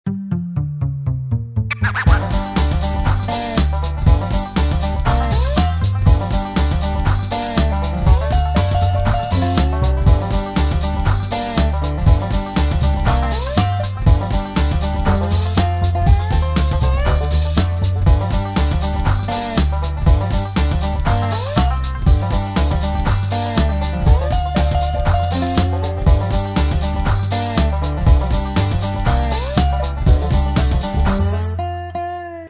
Koillision
Heavy traffic in the garden pond.  Jinli car-pio,  having a happy go.

Artist: Audionautix

Watch My Playlists:   food music nature news world glorious gardens freedom truth, and the American West :